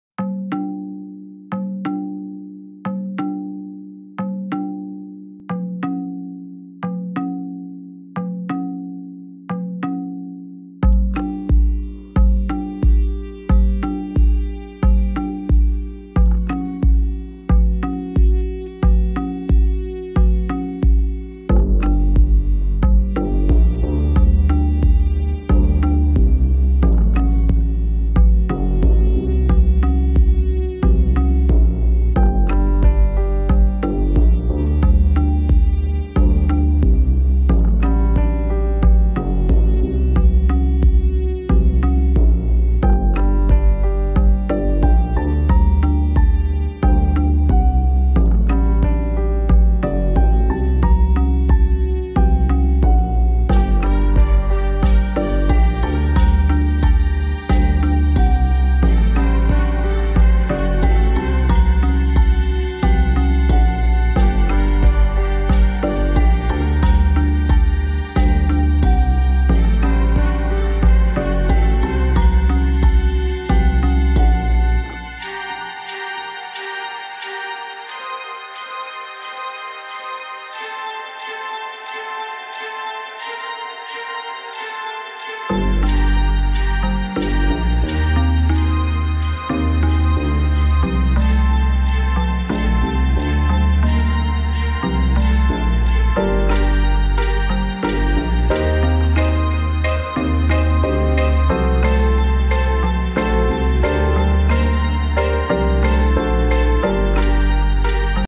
Trash Arts ShowReel- June 2020
A montage collection  from some of the work of 2020 and 2019.
Featuring scenes from- Monstrous, Decline, Unwanted, Acting, You Are going to be a star and many short films!